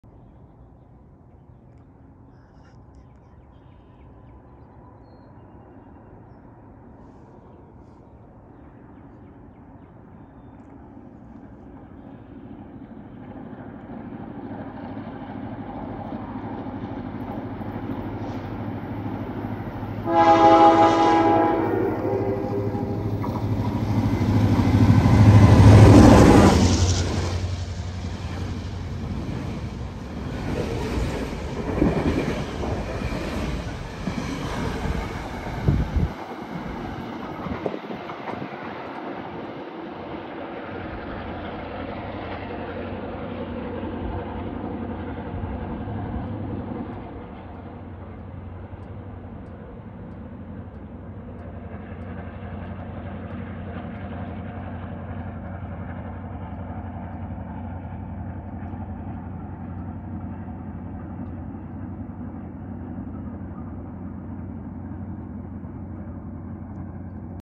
MNRR: GP35R 105 leads an equipment move through Scarborough! Featuring an original BL20GH!
This totally caught me off guard. My first time ever catching a GP35R and it was a good catch too. It was hauling a total of 6 Bombardier cars with BL20GHM 111 trailing behind the GP35R and an original BL20GH 113 trailing in the middle most likely going to be sent for rebuild.

Leading: Rebuilt EMD GP35R 105 (Ex-Reading Company GP35 3637, Conrail 3637)

Trailing: BEC BL20GHM 111

Trailing: BEC BL20GH 113